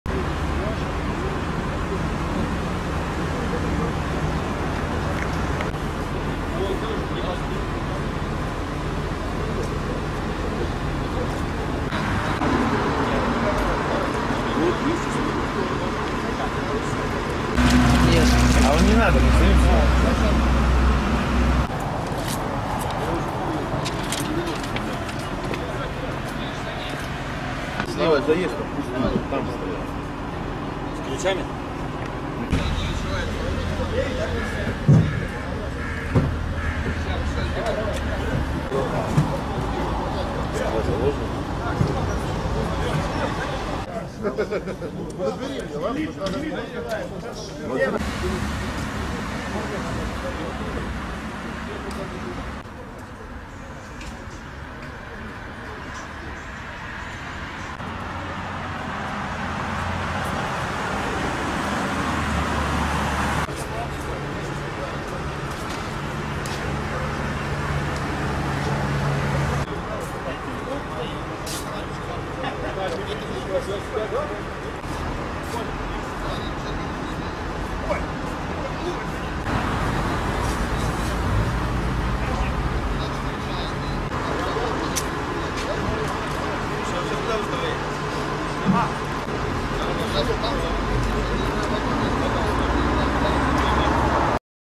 «Рендж Ровер» красногорского стрелка. оперативное видео
«На 26 километре Новорижского шоссе в Подмосковье полиция обнаружила брошенный автомобиль «Рендж Ровер», на котором вчера после совершения убийства в администрации Красногорска скрылся подозреваемый Амиран Георгадзе»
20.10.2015